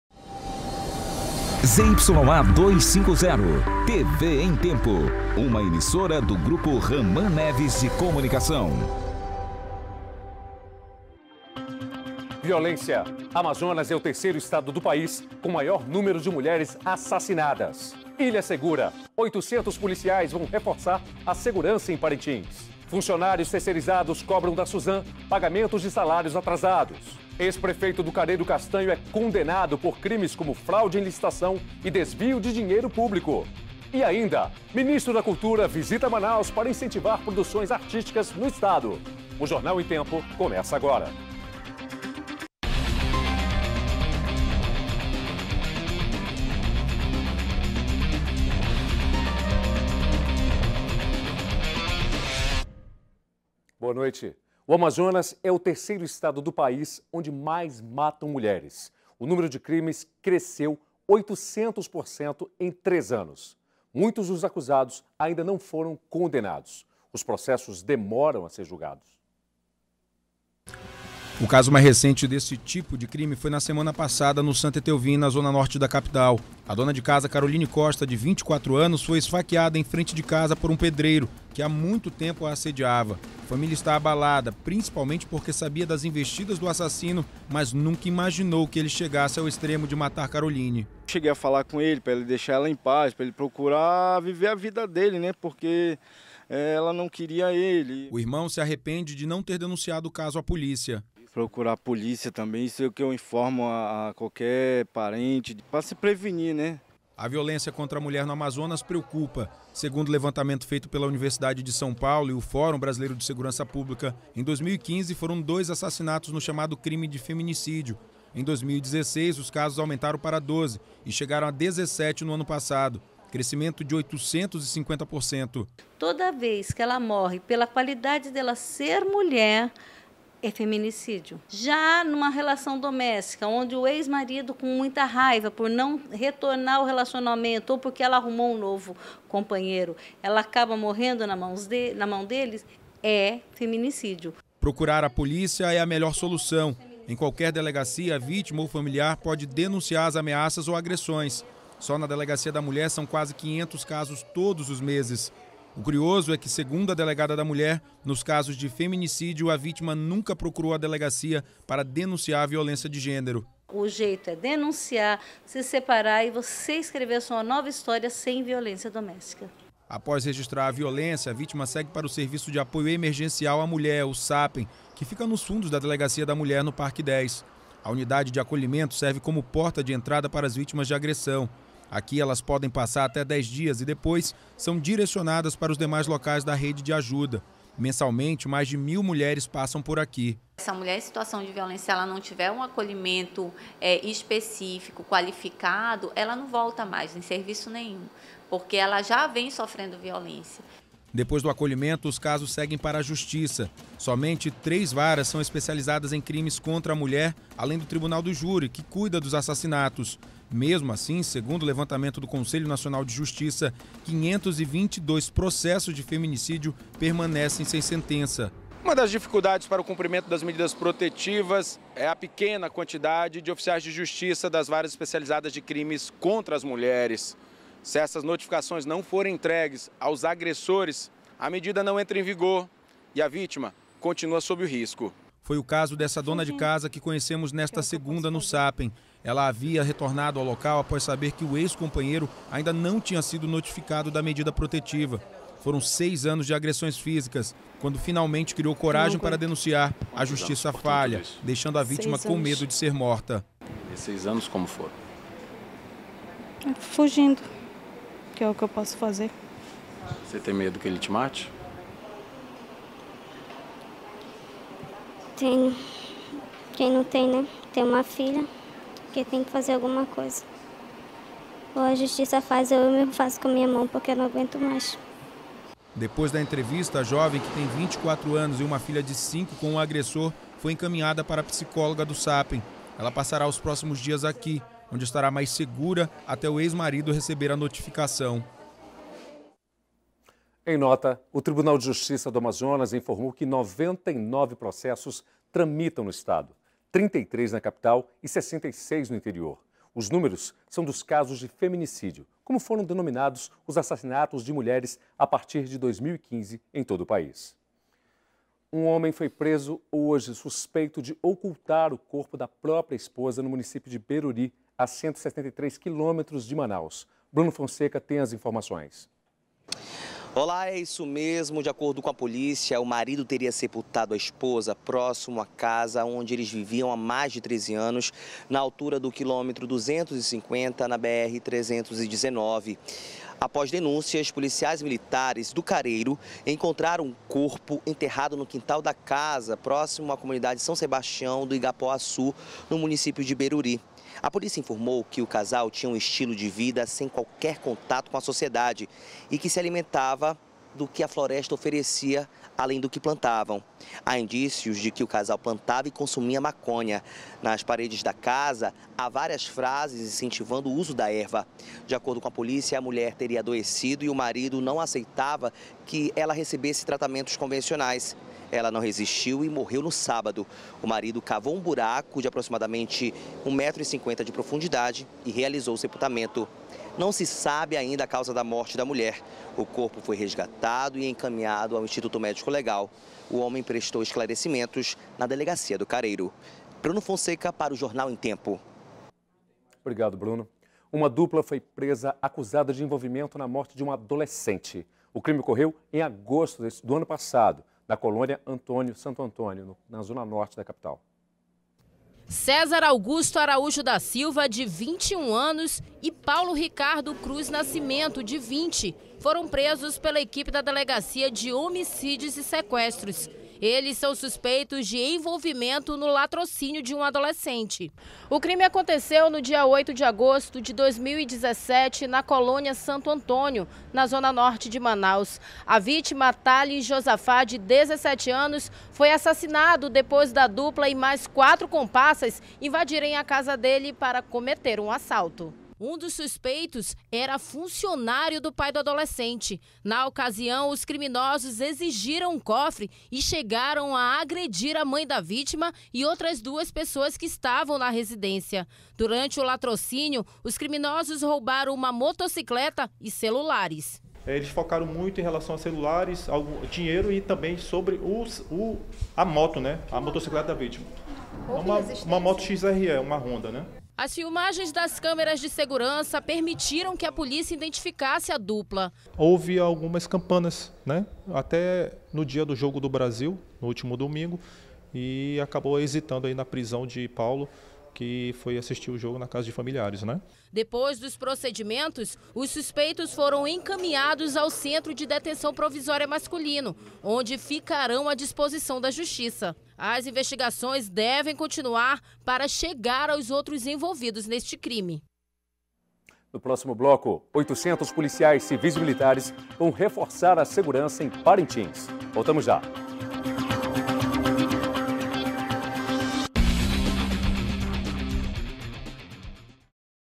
Amazonas é o terceiro estado do país onde mais matam mulheres
01º BLOCO JORNAL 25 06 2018 - Amazonas é o terceiro estado do país onde mais matam mulheres